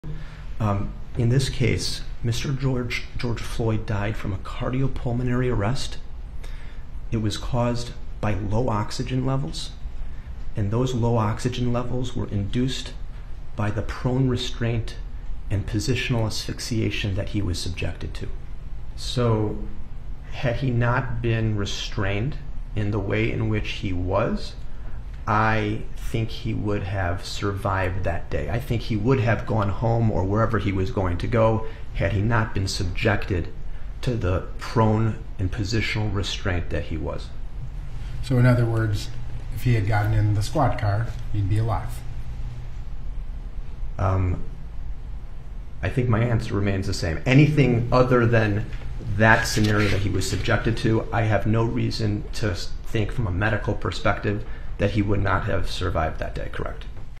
Expert: Floyd would be alive if not for restraint
Dr. Jonathan Rich, a Northwestern Memorial Hospital cardiologist, echoed other witnesses in saying George Floyd died of low oxygen levels from the way police held him down. More experts testify as Derek Chauvin's trial enters its third week. (April 12)